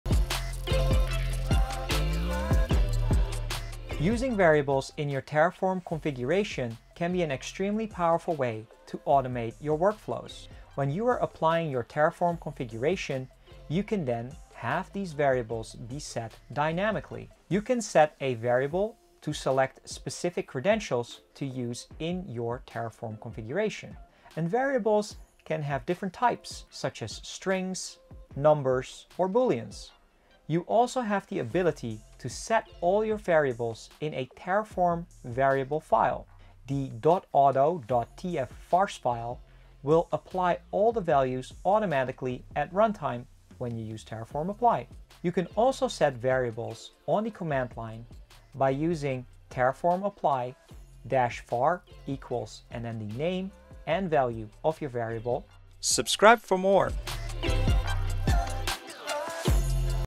Automating Terraform Workflows Using Variables | Terraform Tapas Ep 6
Developer Advocate Mike Elissen explains how companies use Terraform variables automate their workflows.